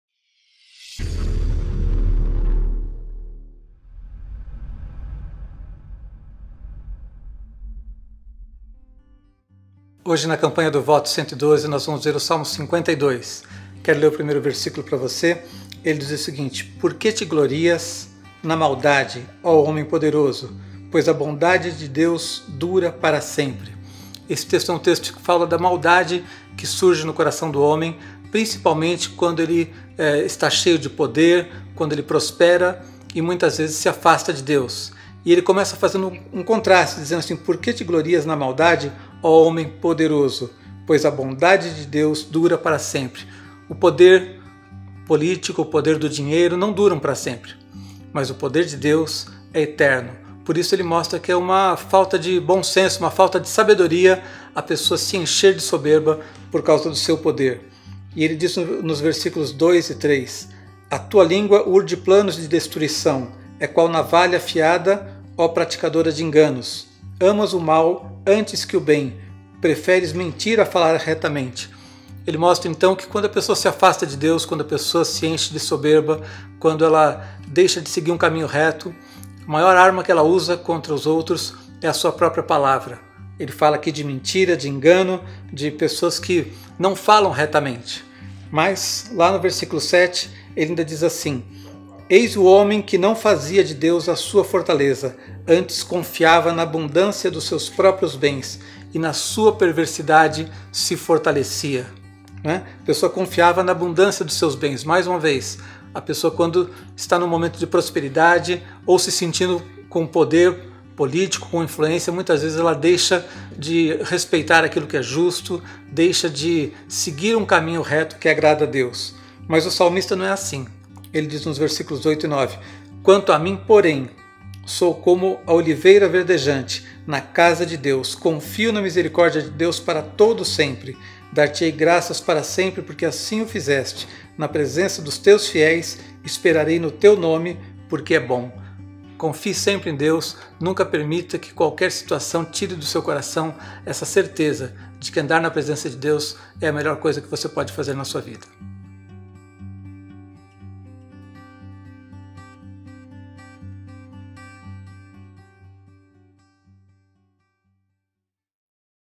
Voto 112 | Salmos 52
Salmo 52

Por que te glorias na maldade, ó homem poderoso? Pois a bondade de Deus dura para sempre.
A tua língua urde planos de destruição; é qual navalha afiada, ó praticadora de enganos!

Amas o mal antes que o bem; preferes mentir a falar retamente.
Amas todas as palavras devoradoras, ó língua fraudulenta!

Também Deus te destruirá para sempre; há de arrebatar-te e arrancar-te da tua tenda e te extirpará da terra dos viventes.
Os justos hão de ver tudo isso, temerão e se rirão dele, dizendo:
Eis o homem que não fazia de Deus a sua fortaleza; antes, confiava na abundância dos seus próprios bens e na sua perversidade se fortalecia.

Quanto a mim, porém, sou como a oliveira verdejante, na Casa de Deus; confio na misericórdia de Deus para todo o sempre.
Dar-te-ei graças para sempre, porque assim o fizeste; na presença dos teus fiéis, esperarei no teu nome, porque é bom.